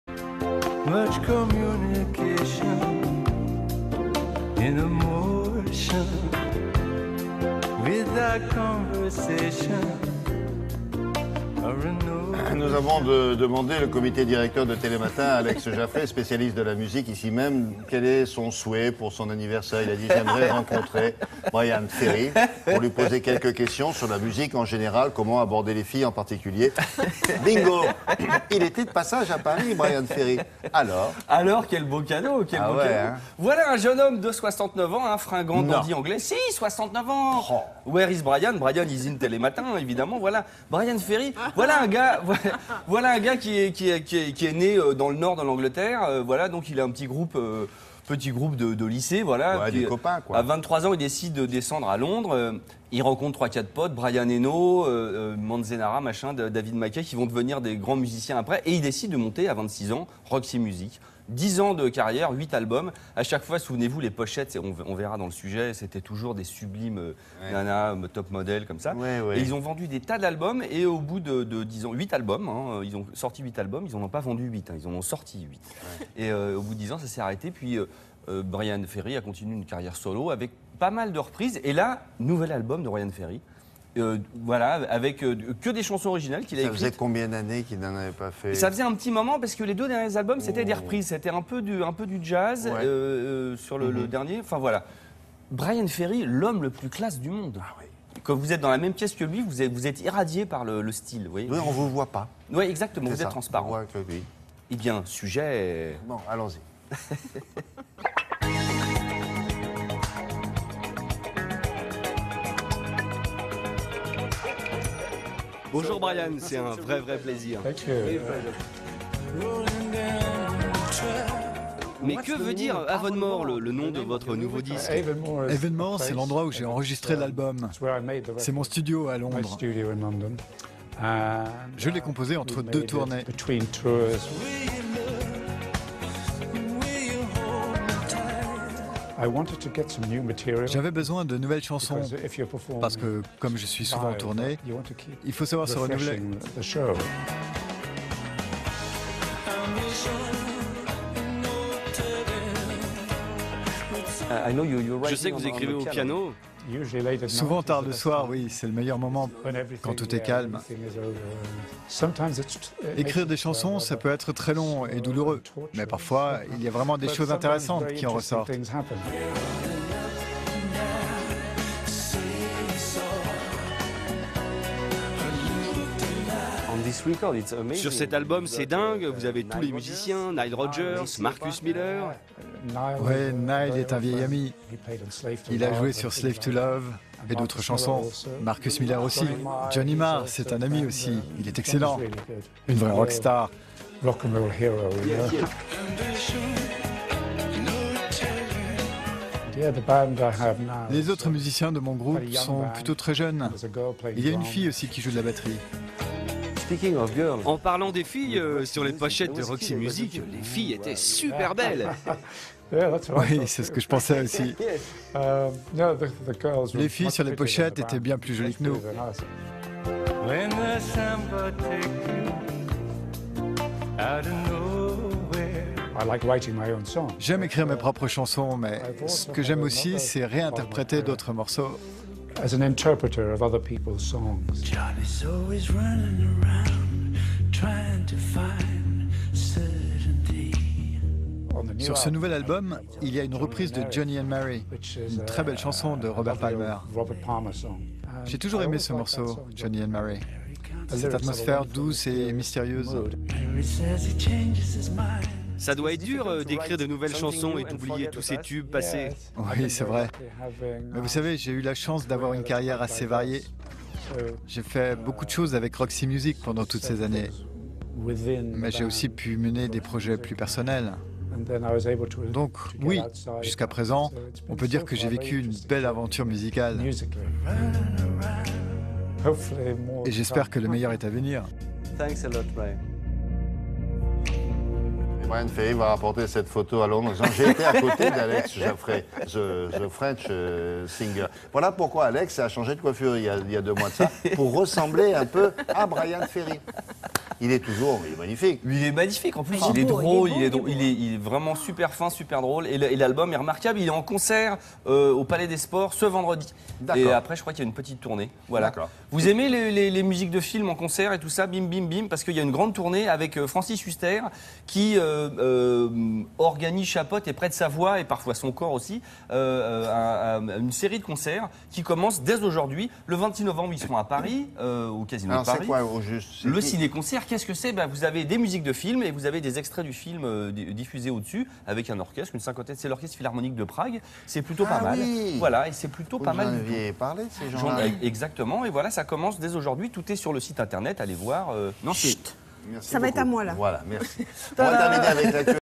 Where is Bryan ?
Pour la sortie de son nouvel album, Bryan Ferry, chanteur et fondateur du groupe légendaire Roxy music, était de passage à Paris. Alex a eu la chance de le rencontrer. De quoi revenir sur la carrière du dandy british, ses succès qui ont fait de lui une légende !